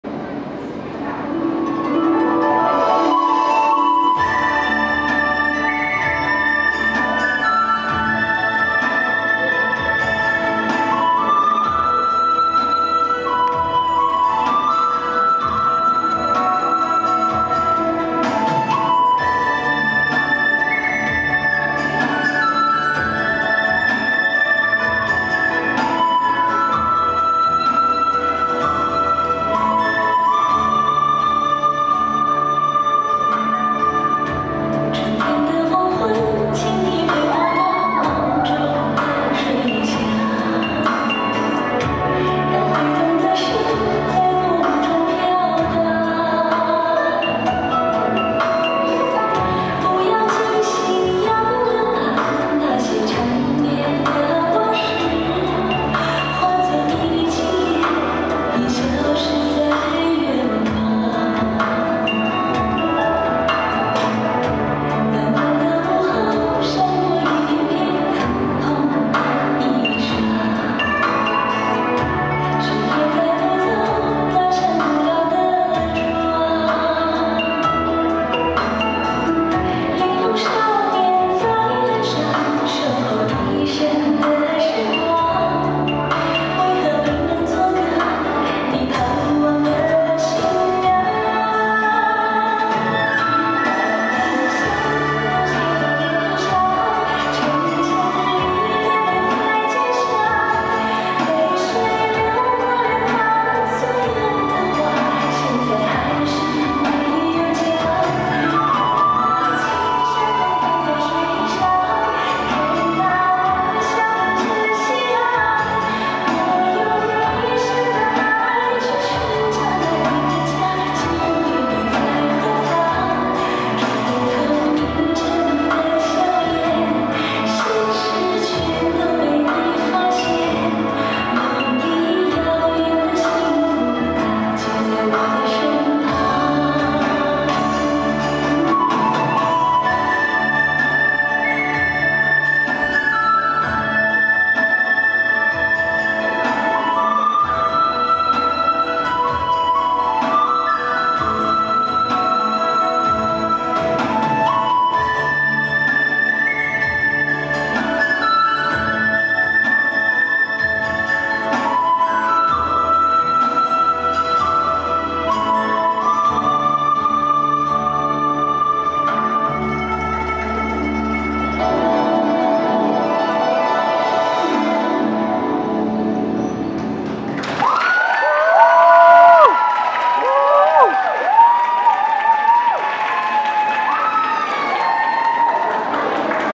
Chinese Umbrella Dance by Concordian International School students in G2 (7 years old)
Chinese Umbrella Dance - CenterStage 8